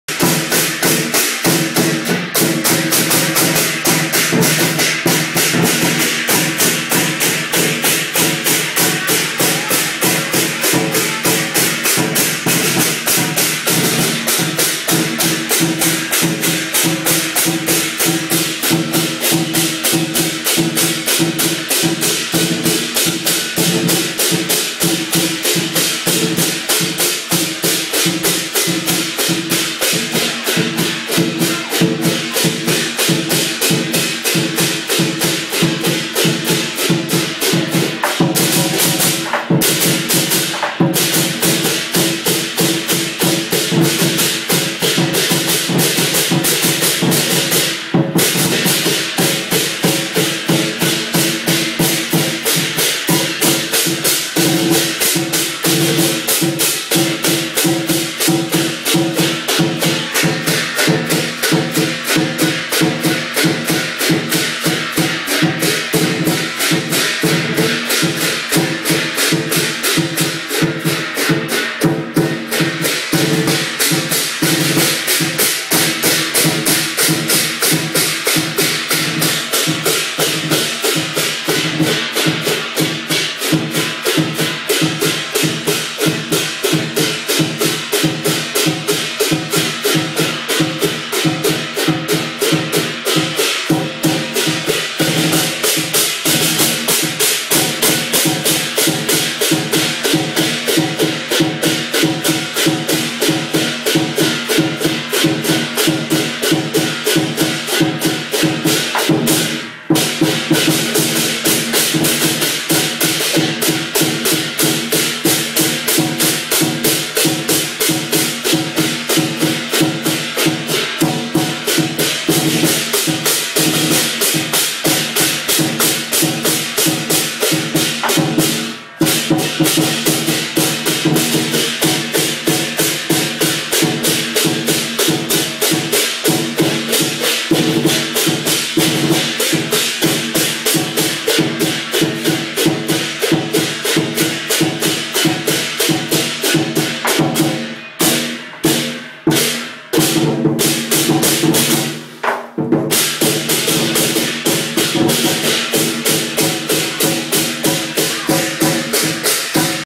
Lion Dance and Fortune God
Lion dance and Fortune god at Paul and Karen's place - an annual tradition